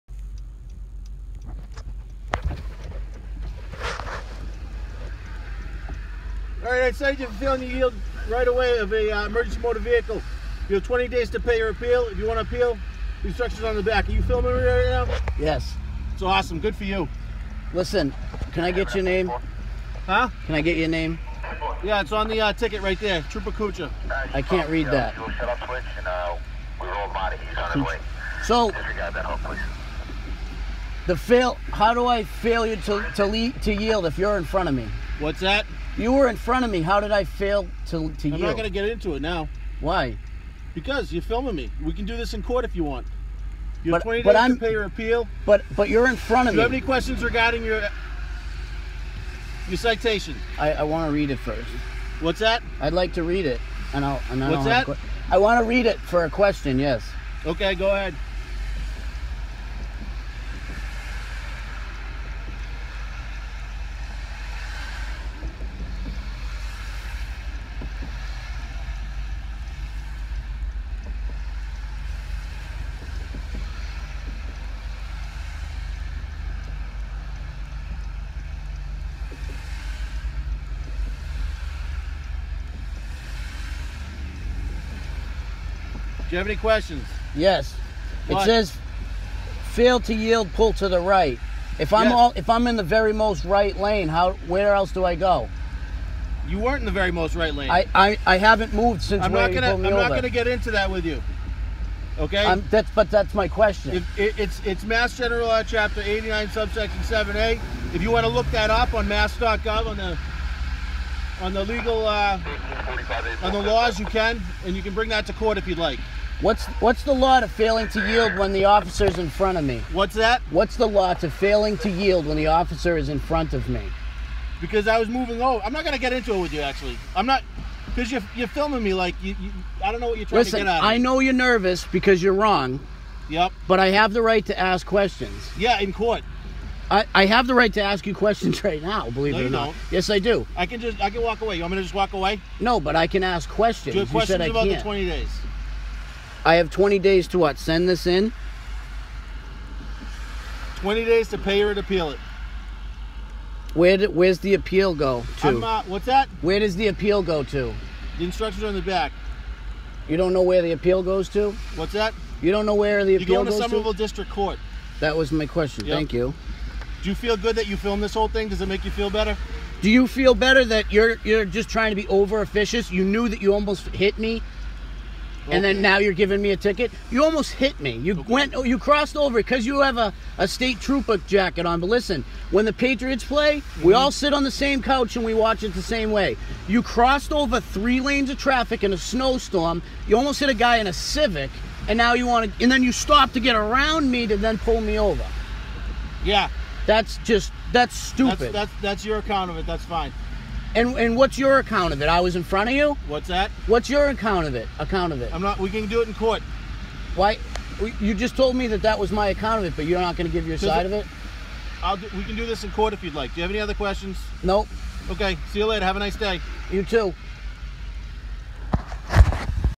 Bonnell gets pulled over
Episode 1:
Bonnell gets pulled over on the way to work in a snow storm. State trooper Kuchy (Coochie) pulls Dan over after crossing 3 lanes of traffic and almost  strikes his vehicle. Attempts to slow down and get behind Bonnell for the "pull over".
Bonnell calmly videos the over officious state trooper for what will be many meme's to come.